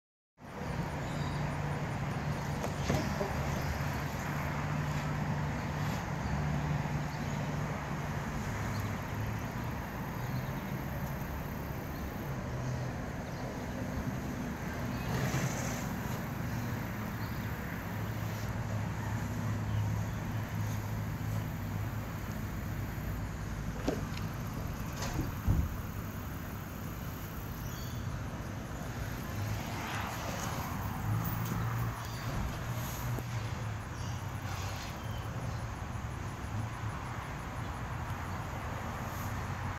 Tembo the elephant plays with logs
Every morning we set up an elephant playground for Tembo. By the time most of our guests get here, the yard looks a mess but that just means Tembo had a lot of fun!